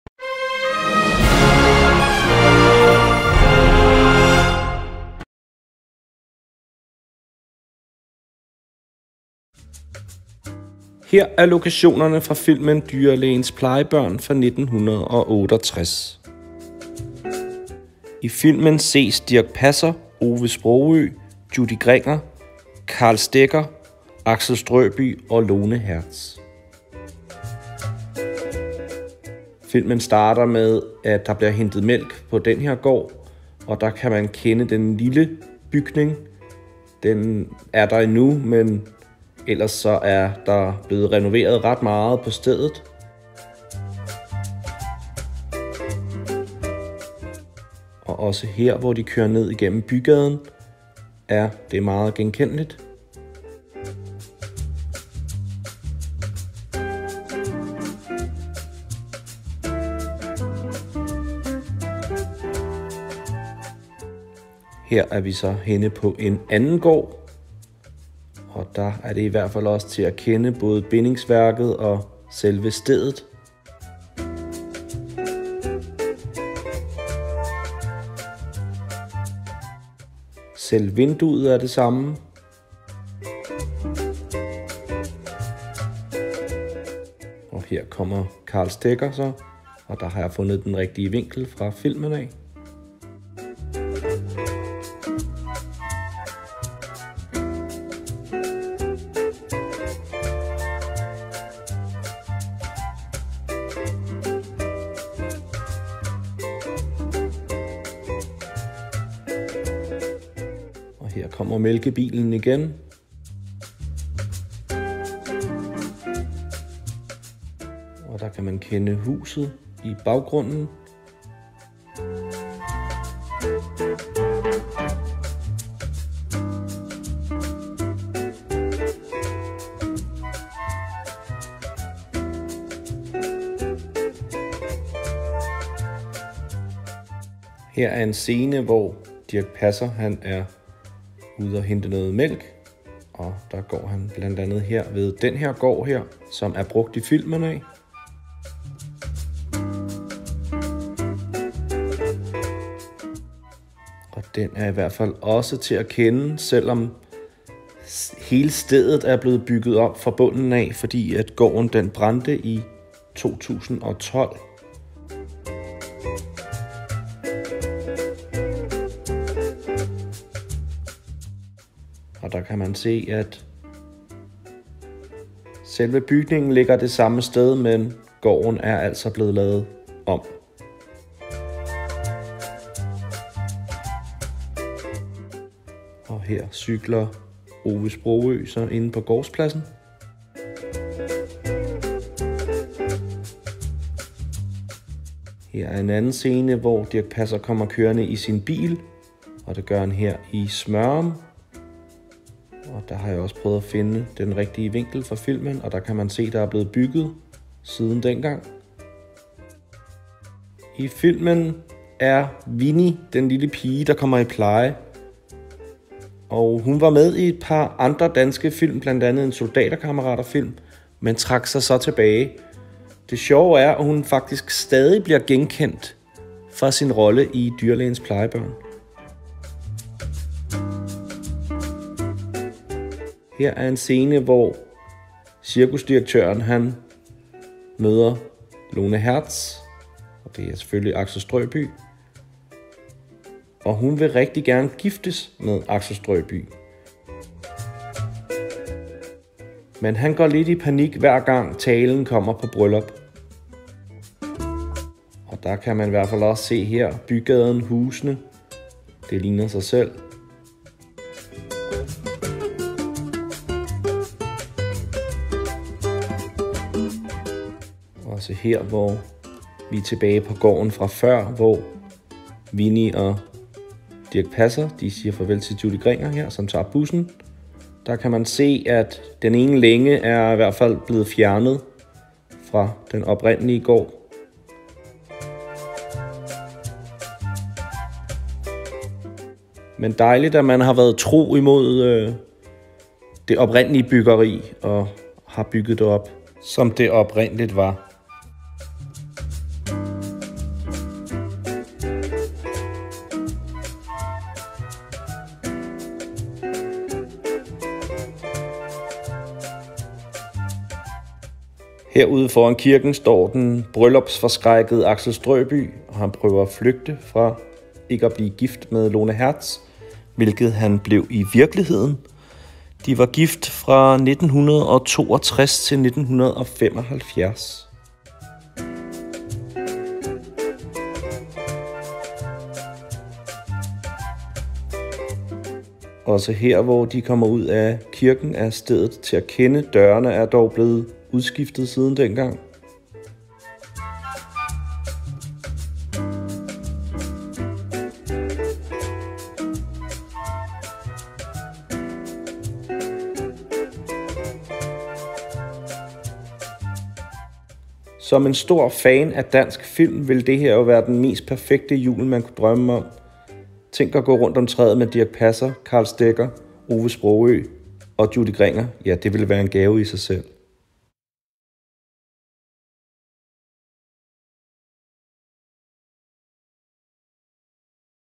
Filmlokationer - Dyrlægens Plejebørn - Julefilm Dirch Passer Ove Sprogøe Judy Gringer .
Stederne hvor filmen fra 1968 , er optaget
Dirch Passer , Ove Sprogøe , Judy Gringer og Karl Stegger